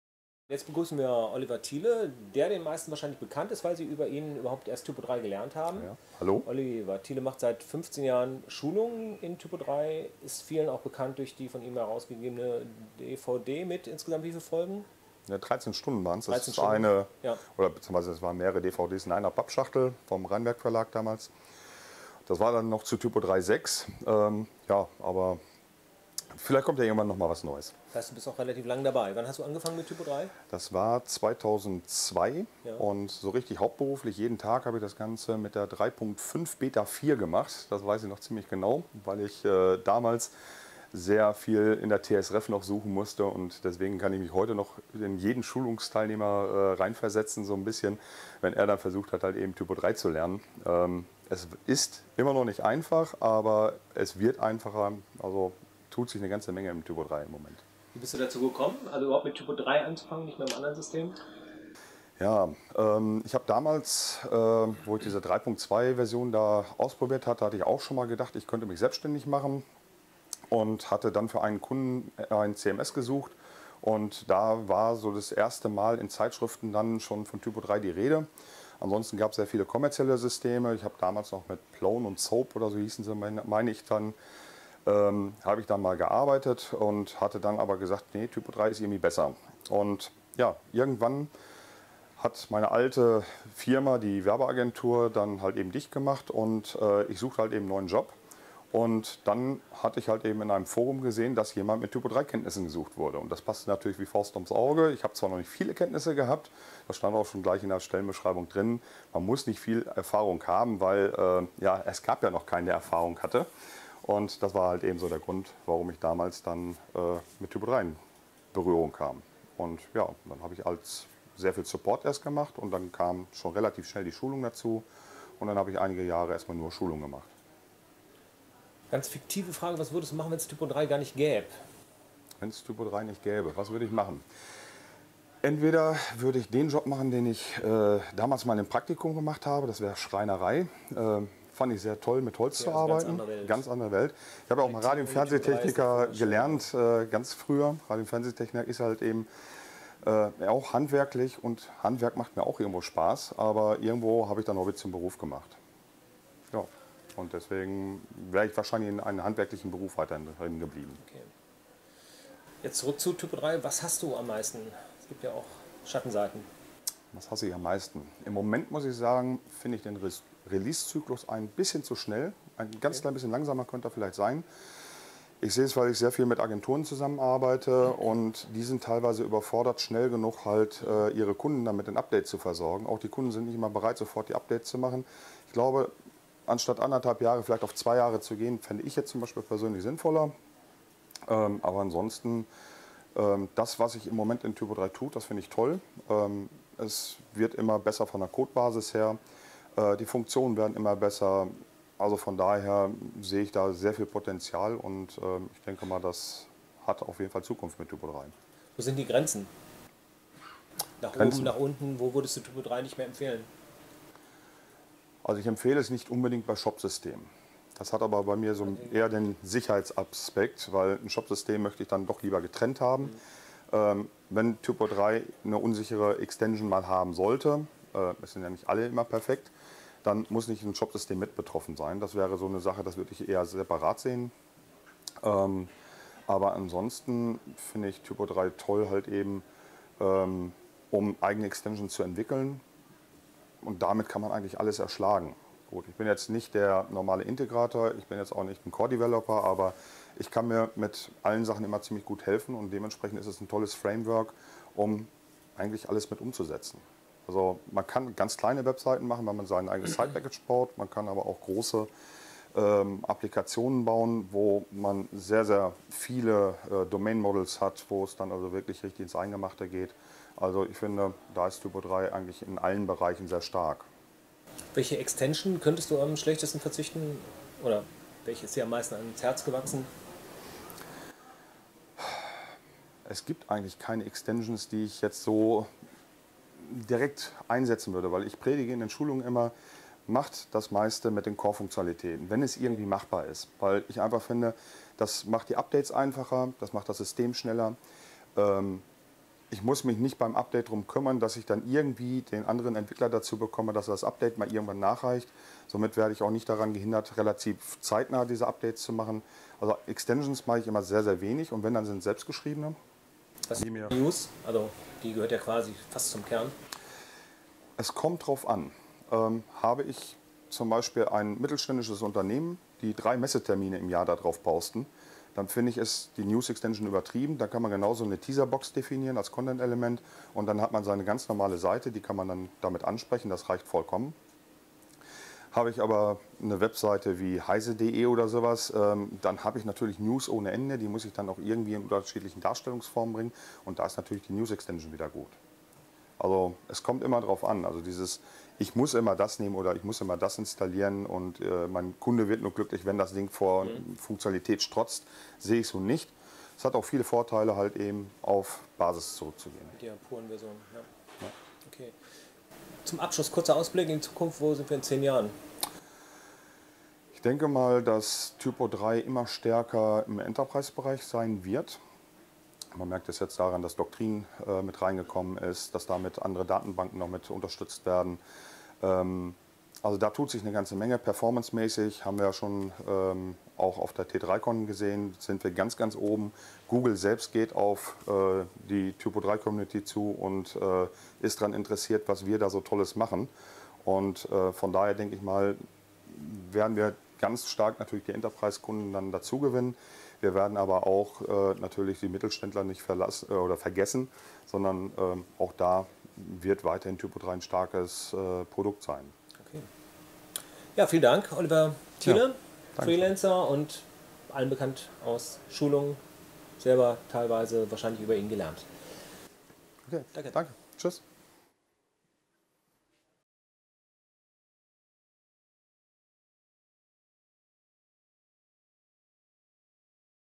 T3CRR im Gespräch mit Oliver Thiele
T3CRR TYPO3 Barcamp in Essen im Unperfekthaus